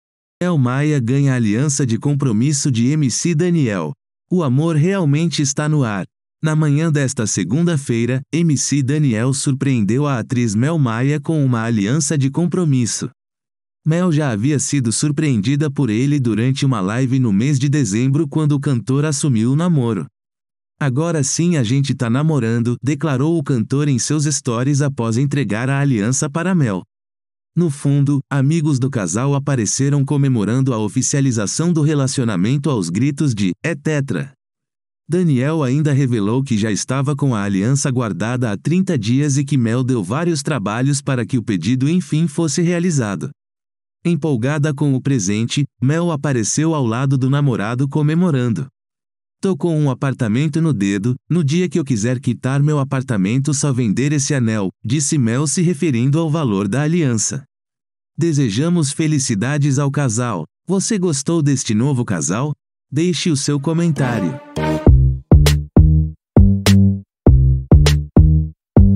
(OFICIAL) - Mel Maia ganha aliança de compromisso de MC Daniel
Mel Maia ganha aliança de compromisso de MC Daniel

O amor realmente está no ar! Na manhã desta segunda-feira, Mc Daniel surpreendeu a atriz Mel Maia com uma aliança de compromisso. Mel já havia sido surpreendida por ele durante uma laive no mês de dezembro quando o cantor assumiu o namoro.

"Agora sim a gente tá namorando", declarou o cantor em seus stories após entregar a aliança para mel.

No fundo, amigos do casal apareceram comemorando a oficialização do relacionamento aos gritos de "É tetra". Daniel ainda revelou que já estava com a aliança guardada há 30 dias e que Mel deu vários trabalhos para que o pedido enfim fosse realizado.

Empolgada com o presente, Mel apareceu ao lado do namorado comemorando.

"Tô com um apartamento no dedo, no dia que eu quiser quitar meu apartamento só vender esse anel", disse Mel se referindo ao valor da aliança.

Desejamos felicidades ao casal! Você gostou deste novo casal? Deixe o seu comentário.